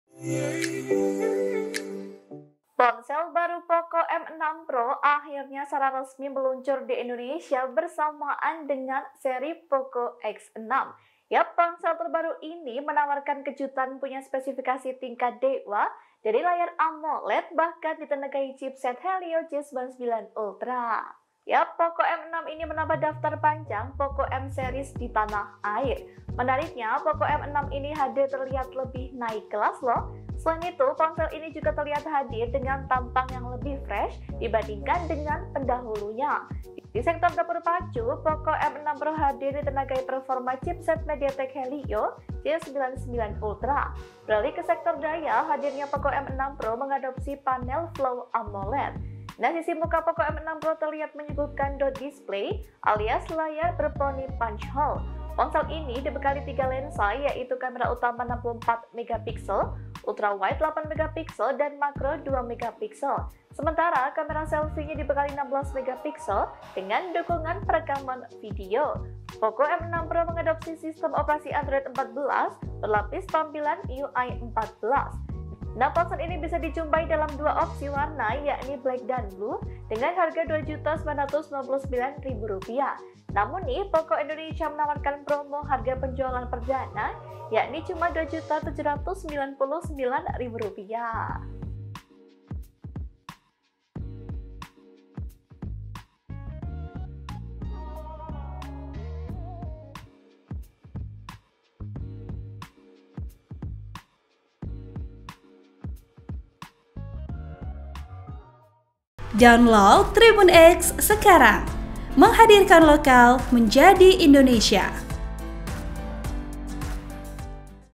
POCO M6 Pro Meluncur, Hadir dengan Layar Amoled dan Helio G99 Ultra, Harga Murah Cuma Rp 2 Jutaan!
TRIBUN-VIDEO.COM - Ponsel baru POCO M6 Pro akhirnya secara resmi meluncur di Indonesia bersamaan dengan seri POCO X6.

Ponsel terbaru ini menawarkan kejutan punya spesifikasi tingkat dewa, layar AMOLED bahkan ditenagai chipset Helio G99 Ultra.

POCO M6 ini menambah daftar panjang POCO M Series di Tanah Air.

(Tribun-Video.com/Tribunshopping.com)

Artikel ini telah tayang di Tribunshopping.com dengan judul "POCO M6 Pro Turut Hadir di Indonesia Bawa Banyak Peningkatan, Ini Spesifikasi dan Harganya"
Host: Feba